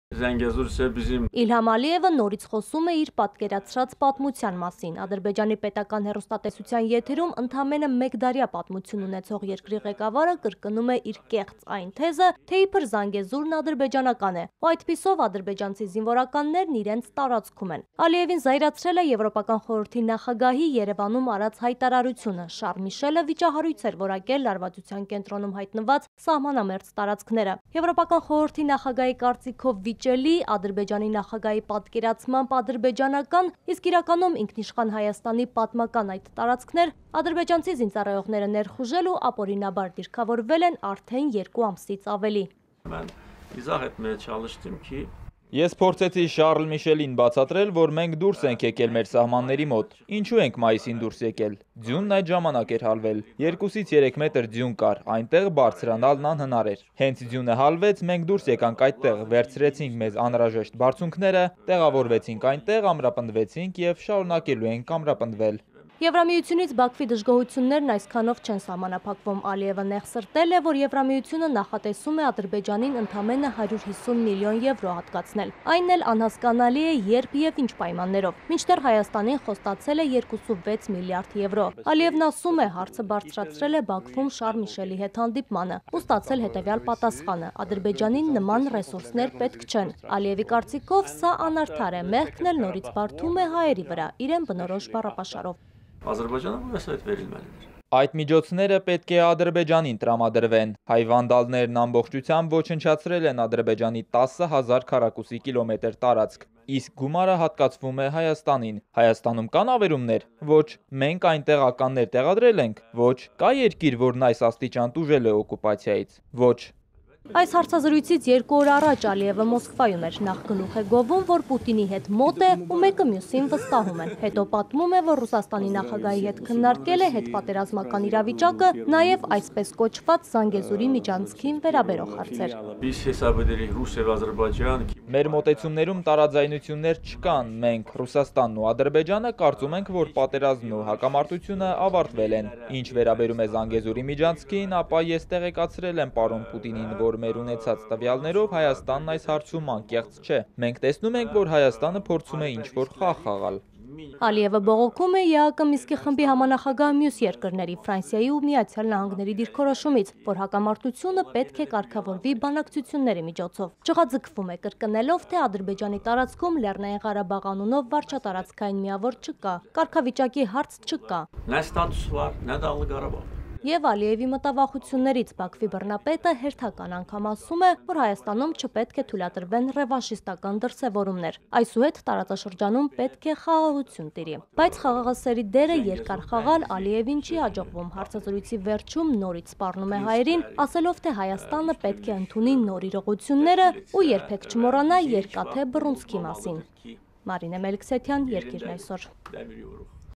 Ընդամենը մեկդարյա պատմություն ունեցող երկրի նախագահը կրկնում է թեզը, թե Զանգեզուրն ադրբեջանական է
Ընդամենը մեկդարյա պատմություն ունեցող երկրի նախագահը կրկնում է այն կեղծ թեզը, թե իբր Զանգեզուրն ադրբեջանական է